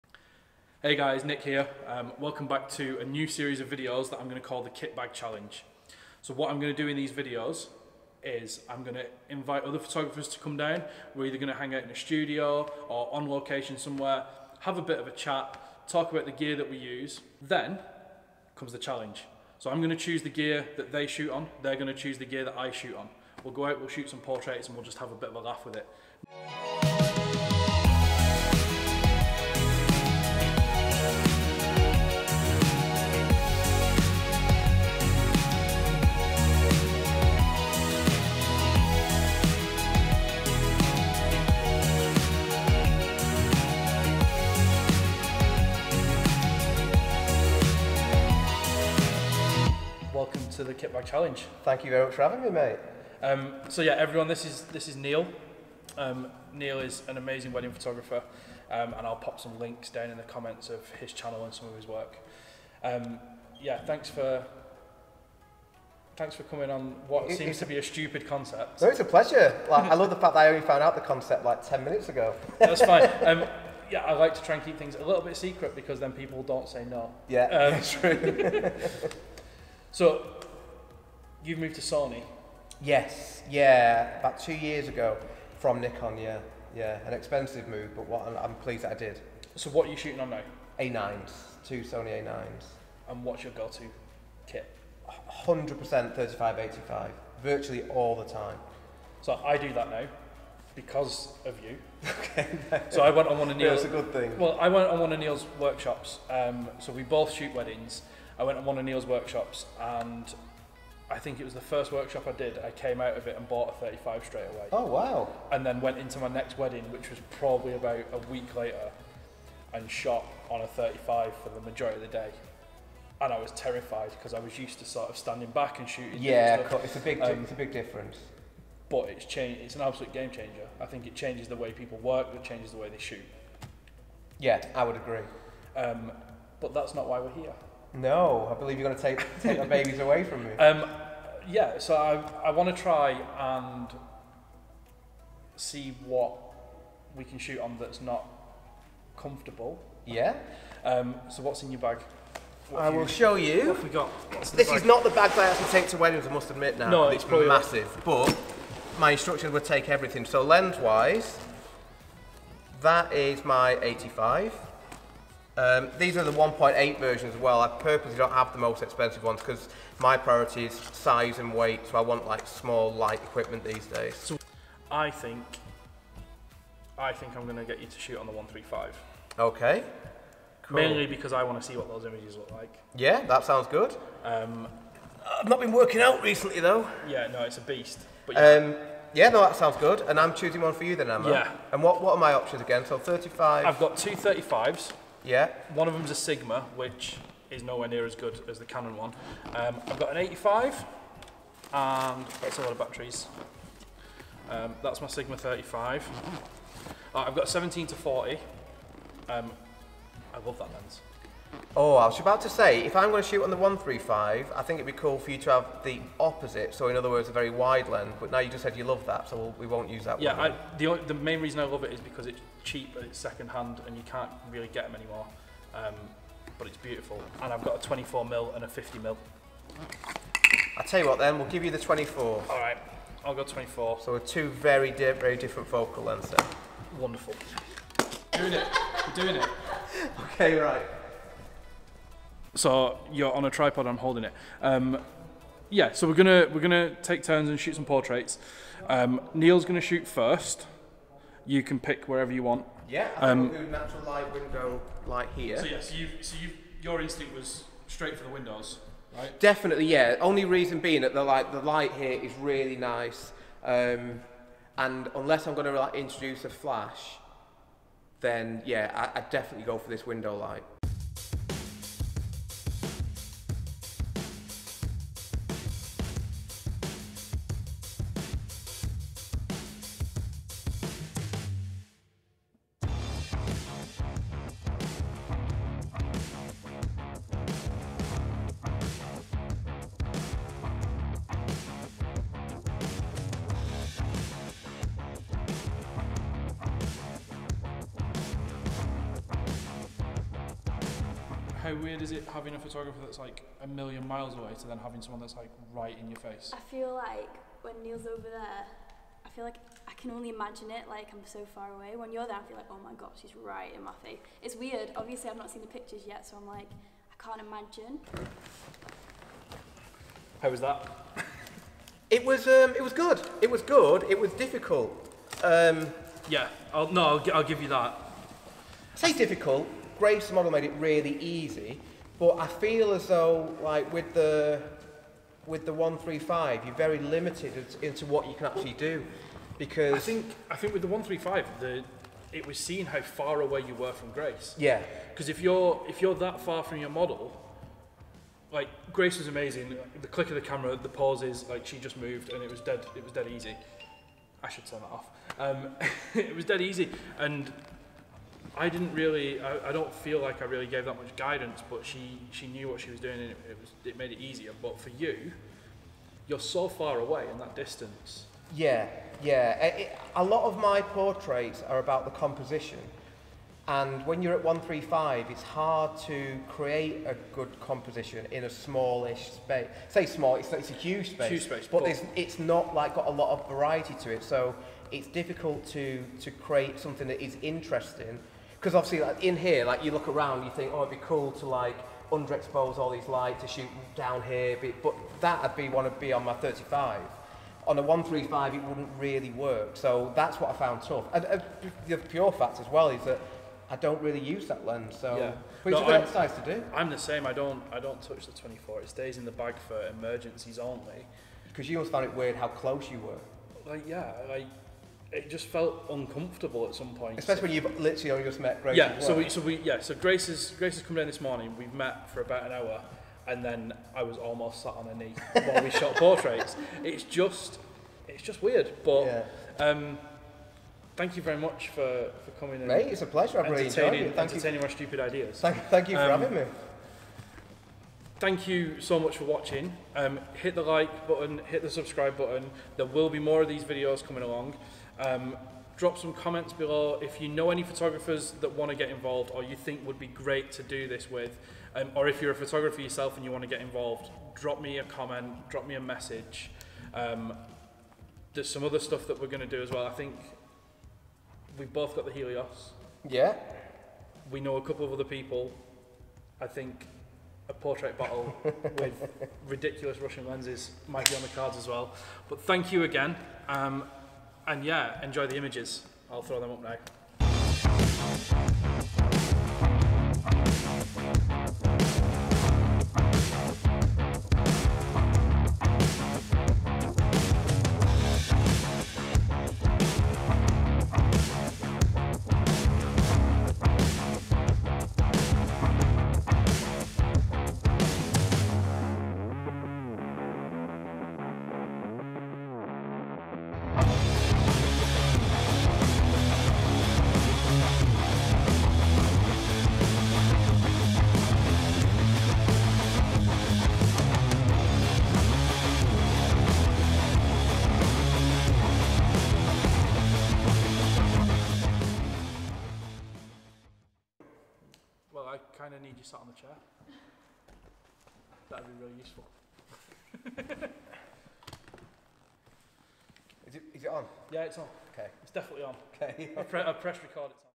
Kitbag Challenge with Neil Redfern
In this new series of videos I will be inviting other photographers to come along on a shoot where we pick each others kit to shoot on.